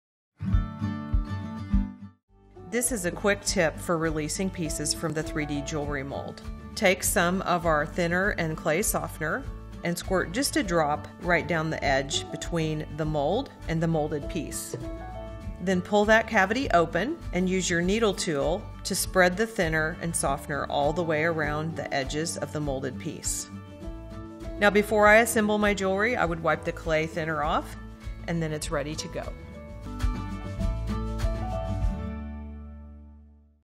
Using Sculpey Clay Softener & Thinner to Release Pieces from 3D Jewelry Mold
This helpful tip shows you how to get your baked Liquid Sculpey pieces out of the Sculpey 3D Jewlery Mold.